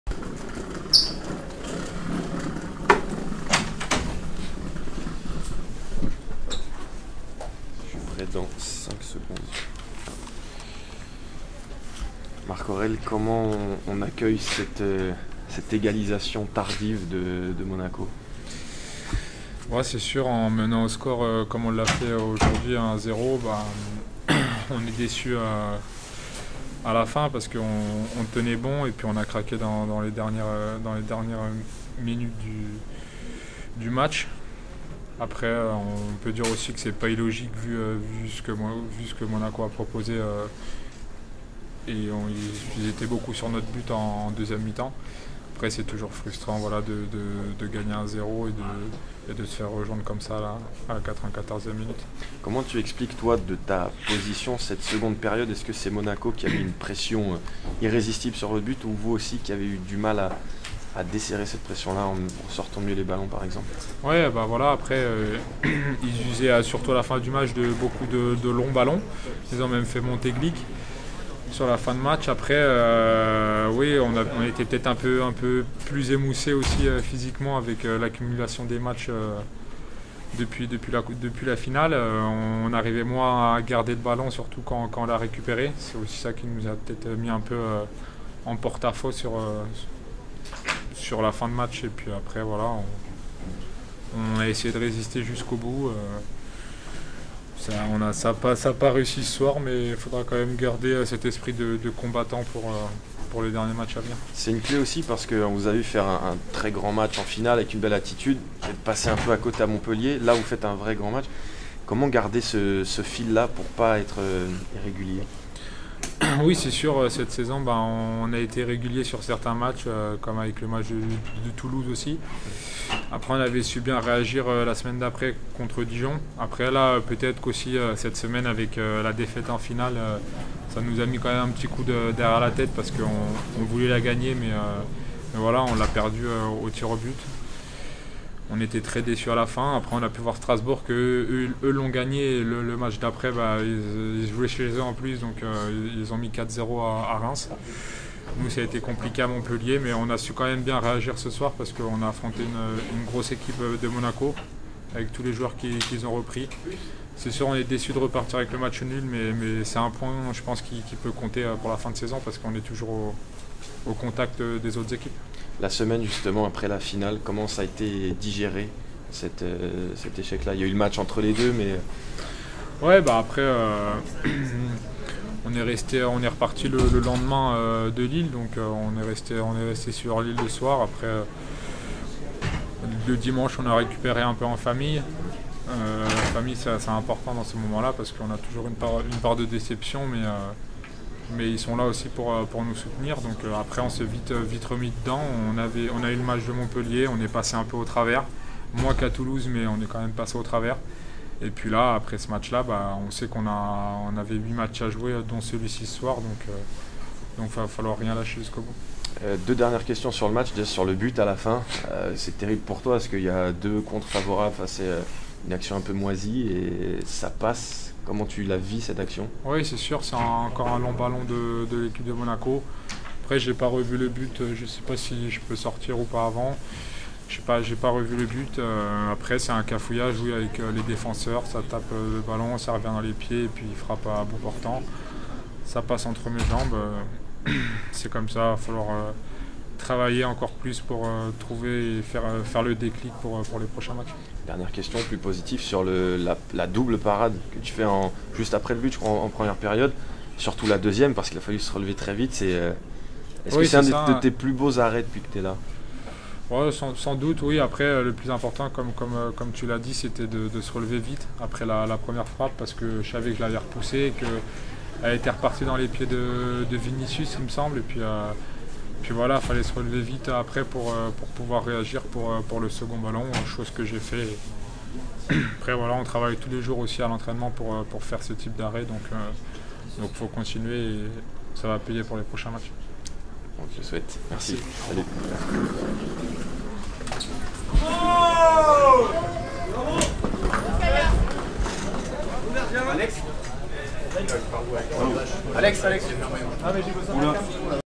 MARC-AURÈLE CAILLARD RÉAGIT APRÈS GUINGAMP - MONACO (1-1) / Ligue 1 - 6 avril 2019
L'En Avant de Guingamp a été rejoint au score dans les dernières secondes du temps additionnel. Stevan Jovetic a égalisé, profitant d'un cafouillage dans la surface. Il répond à Félix Eboa Eboa, auteur d'une tête à la 23e minute.

Le partage des points est logique dans cette rencontre de la 31e de journée de Ligue 1 de football, où chaque équipe a gagné une période. Il n'en est pas moins rageant pour Guingamp, dernier de Ligue 1, malgré la défaite caennaise face à Nîmes.

Merci pour vos encouragement à coup de pouces bleus et de commentaires, qui améliorent aussi le référencement ;) 👍

STATISTIQUES

37 Possessions (%) 63
11 Tirs 18
2 Tirs cadrés 5
60 Passes réussies (%) 80
2 Hors-jeux 1
3 Corners 9
14 Centres 37
17 Fautes 14
4 Cartons jaunes 5
0 Cartons rouges 0

LES AUTRES RÉACTIONS

Bientôt...

REJOIGNEZ-MOI !